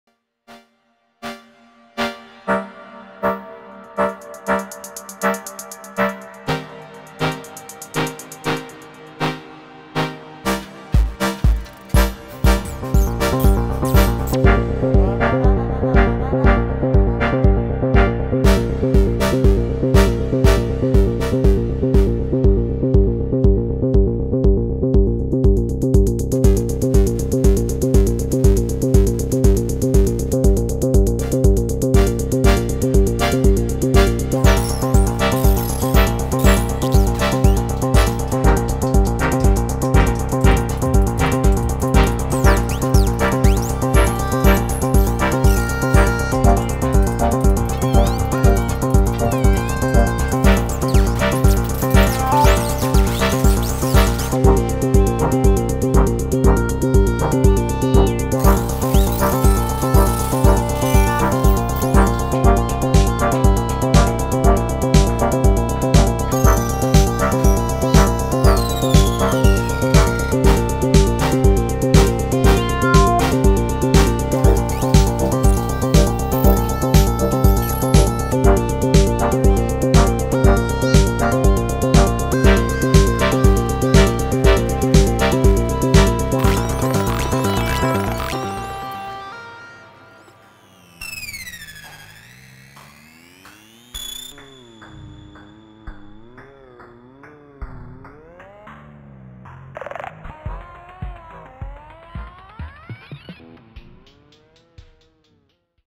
vcp challenge #62
for this edition, the challenge is to make a patch with instruō saïch